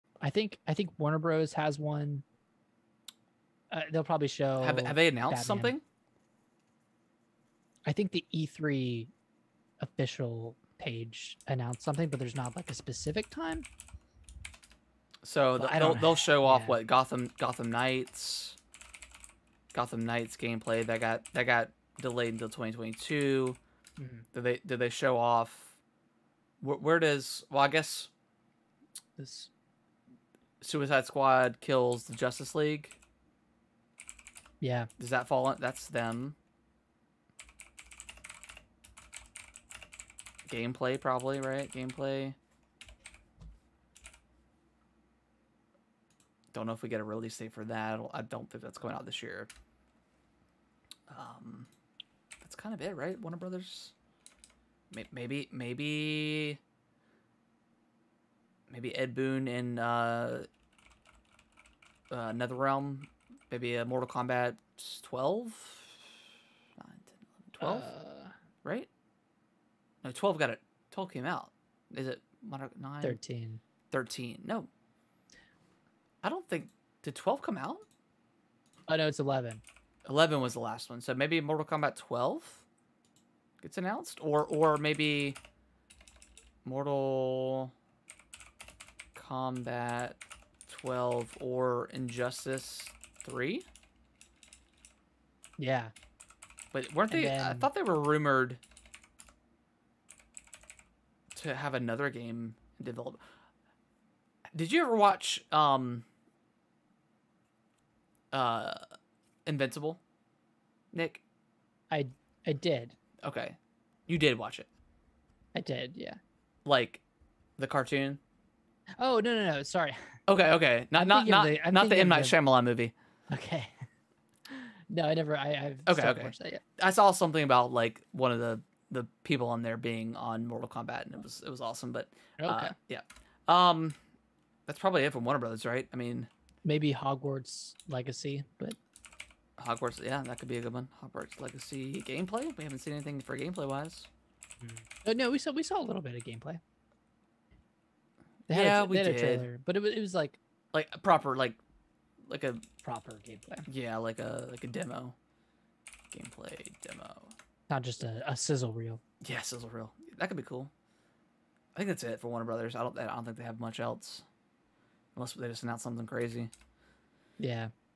Warner Bros 2021 E3 Predictions!
My brother and I talk about our predictions for Warner Bros. This segment was taken out of our weekly gaming podcast called "Gaming with the Broscast."

Listen to the show on all podcasting platforms every Monday